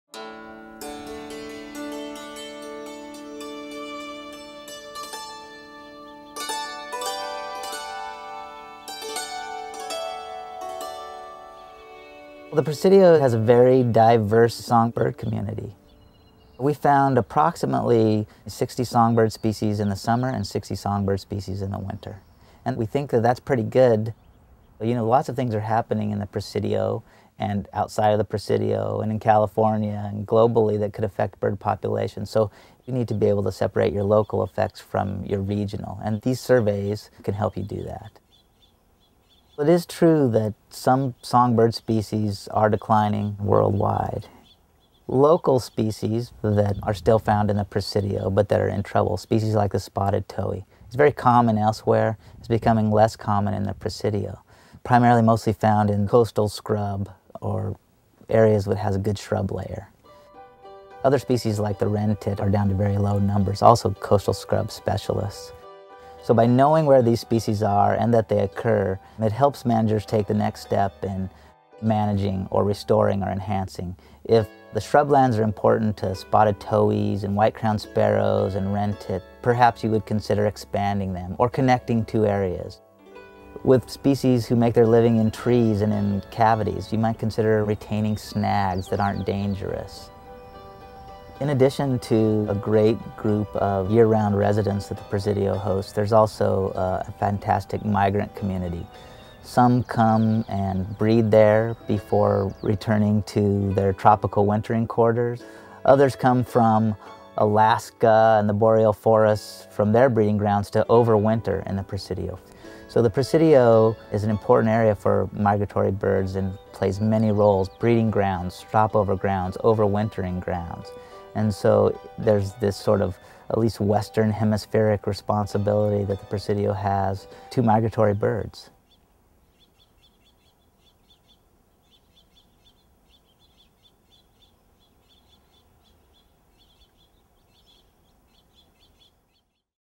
Oasis: Songbirds
The Presidio Trust has prepared a series of short films highlighting ecology in the Presidio and efforts to identify, monitor, and conserve flora, fauna, and natural systems. The Presidio's diverse landscape protects the most complete remaining mosaic of natural systems in San Francisco, with a high density of rare and endangered plants, and an unusually high diversity of invertebrate and wildlife species for a park in an urban location. The Presidio is also a very human place where people and nature thrive in harmony. This film is by Melissa Peabody.